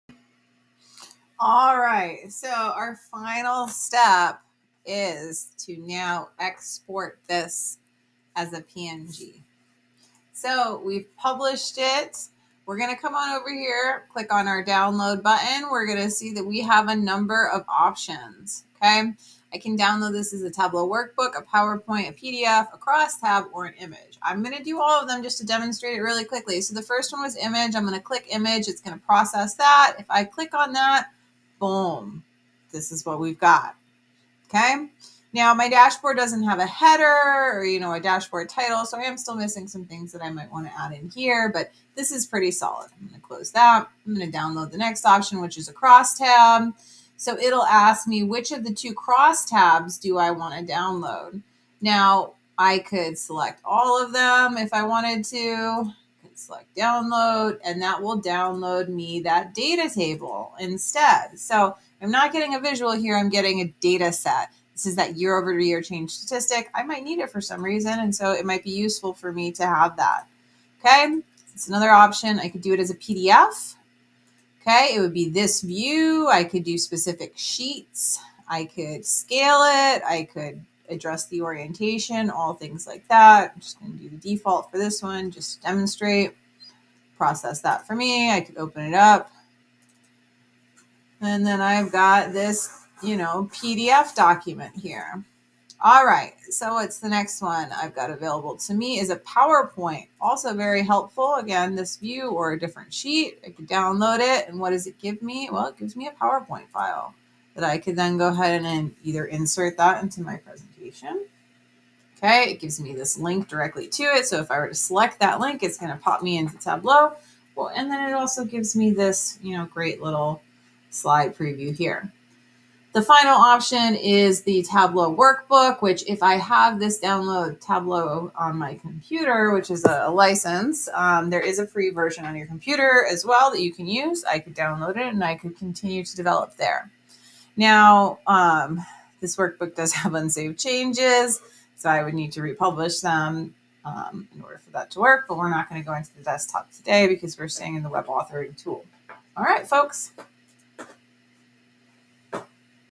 CCV CIS-2180 - Module 4 - Part Five - Exporting Finished Products from Tableau
In this final demonstration video for Module 4 assignment I show you how to export your finished dashboard and other items from Tableau web Authoring for use in other tools.